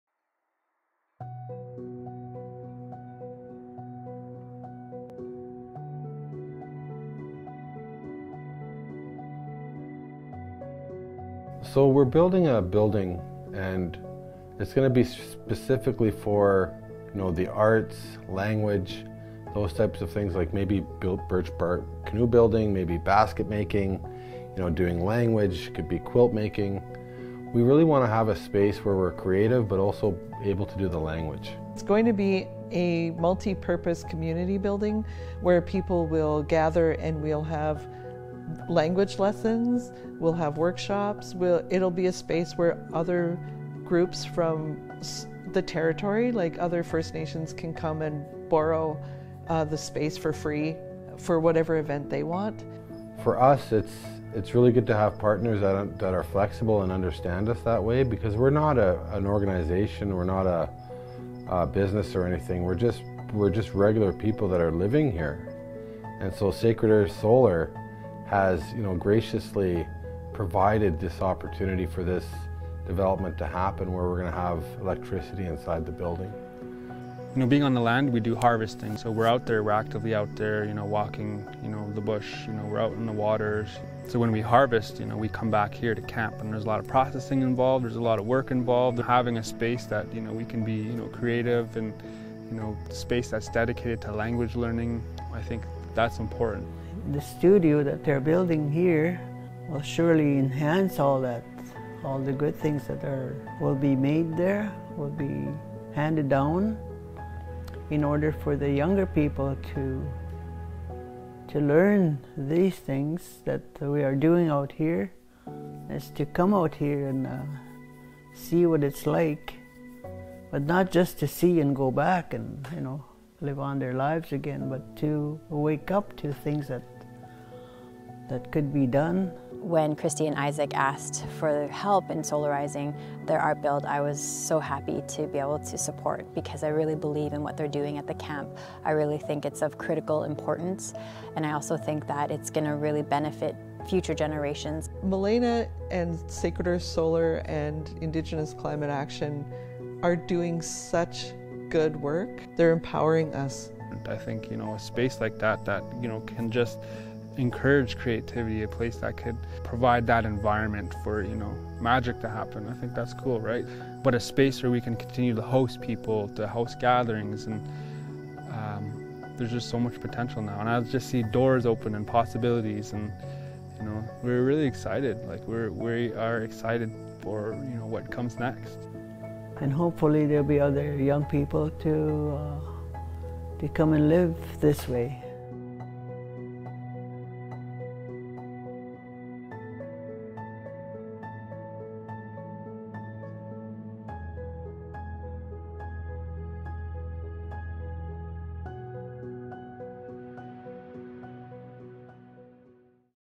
Part 2 of 4: New Building - Nimkii Aazhibikong Solar Series
Fresh Shift Productions documented the process of solarizing the buildings at Nimkii Aazhibikong with the support of Indigneous Climate Action and Sacred Earth Solar. This is the 2nd in the series of four videos documenting the process.

*Nimkii Aazhbikong is an Anishinaabeg intergenerational language and land-based practices camp that is youth-led, Elder directed, and community supported. The language and art studio at Nimkii Aazhibikong was given the name Waaseyaawin Teg, or 'our culture coming to light,' by Elder Linda Toulouse.

The solarization of Waaseyawiin Teg began as a dream almost three years ago. The solarization is now complete, and the off-grid language and art studio is almost entirely powered by the sun.

Nimkii Aazhibikong partnered with Indigenous Climate Action and Sacred Earth Solar to implement solar power as a transition technology on the way back to living completely sustainably with the land.

Standing in solidarity with each other allowed the seedling of a dream planted three years ago to sprout into being.

We would like to thank:
Elders Mary Wemigwans, Linda Toulouse and Larry McLeod, and all who have come to be a part of Nimkii Aazhibikong and who built the studio.

We also acknowledge Melina Laboucan-Massimo, Christi Belcourt and Isaac Murdoch whose conversations and dreams led this project to reality.

We thank Steve with Solar Associates.

And finally but not least, those who also assisted in supporting the solar project and the language and art studio:
Kalliopeia Foundation, Seventh Generation Fund, Joyce Dale, Patrick Cameron and NDN Collective.